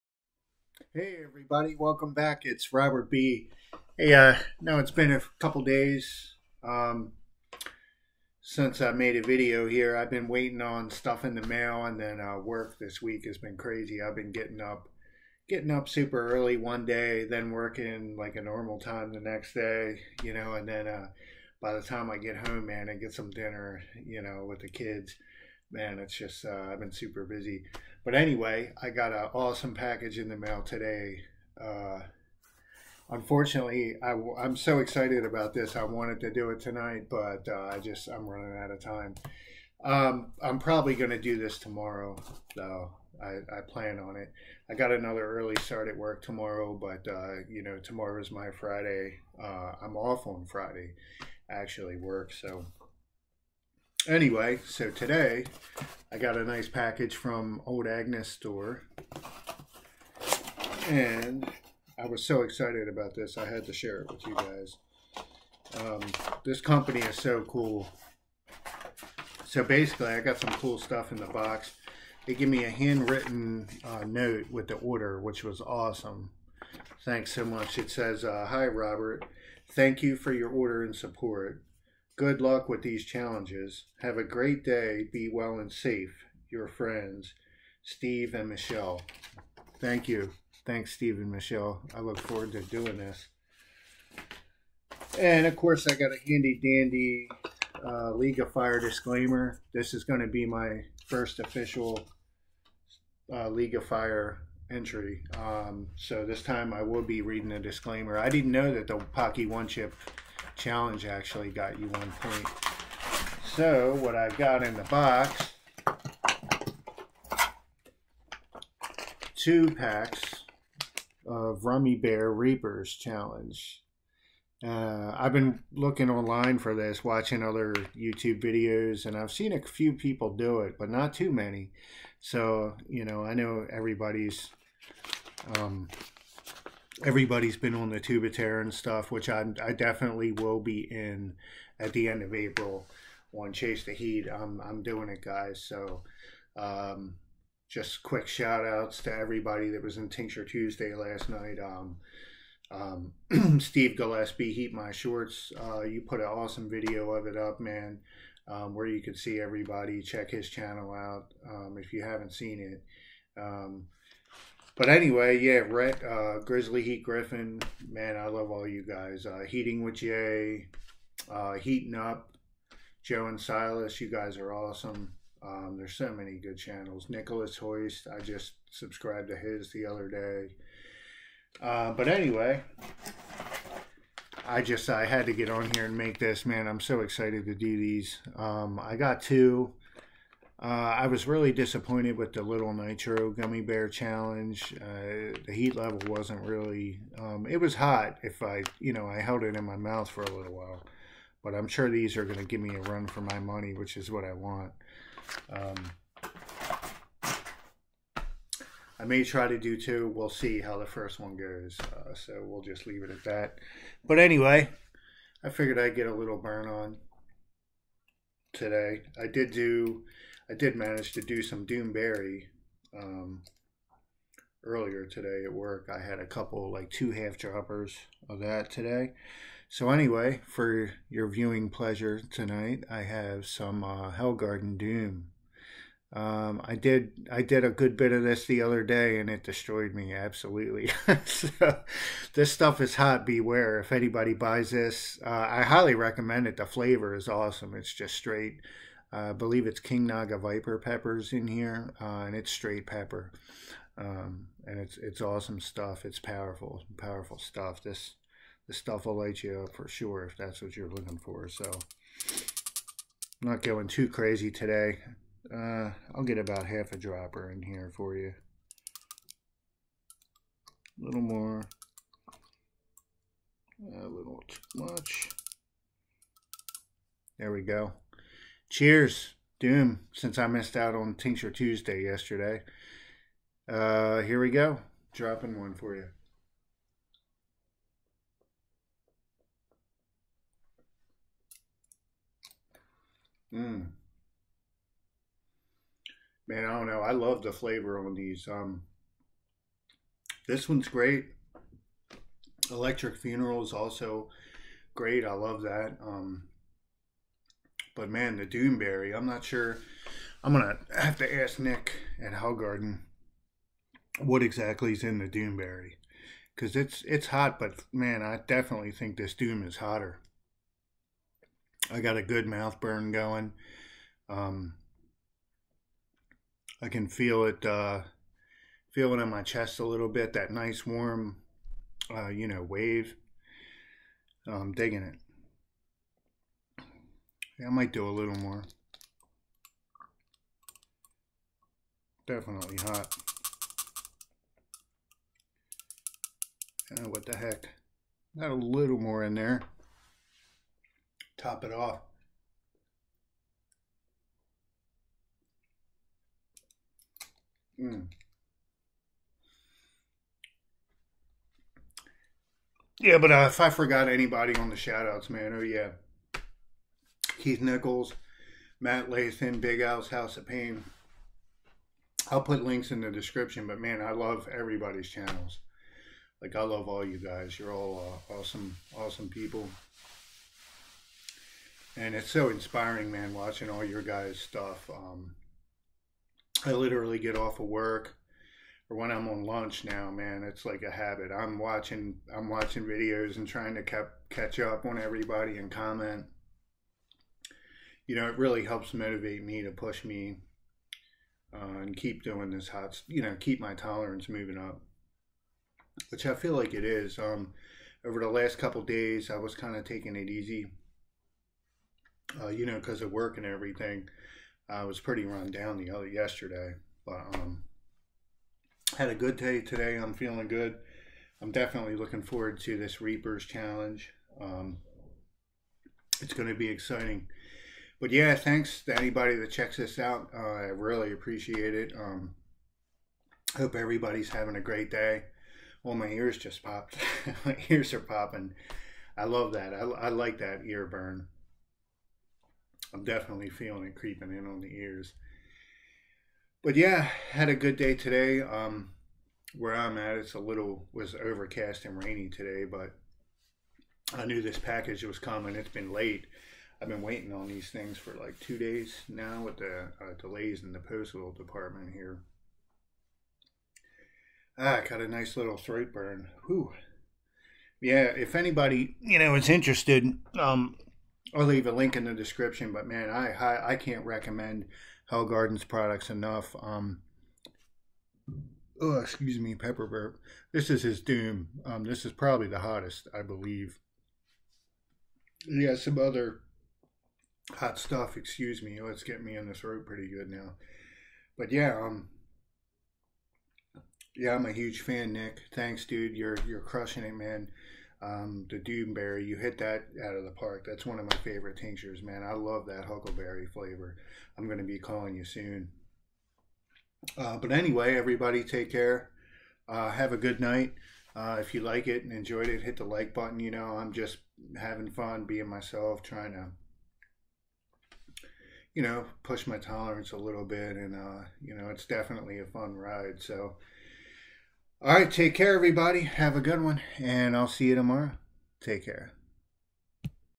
got something hot in the mail :}
today i finally got a package i have been waiting for!!! and i drop some hellgarden doom tincture for your viewing pleasure!!
take care everyone, and burn on:}

also check out these awesome youtube channels!! i love all of these